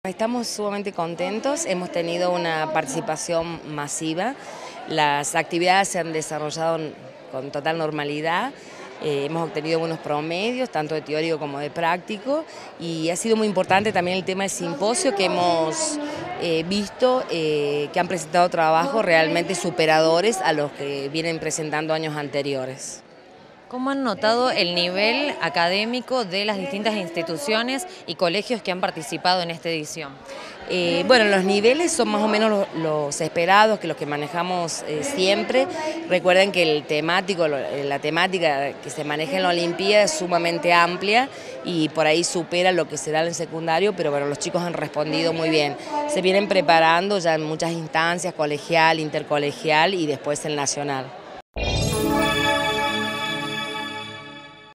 OLIMPIADA ARGENTINA DE BIOLOGÍA - GRACIELA RAFFAINI – INTEGRANTE COMITÉ ORGANIZADOR
BALANCE DE LA 26° EDICIÓN DEL CERTAMEN NACIONAL
Facebook.com/uniriotv
Twitter.com/uniriotv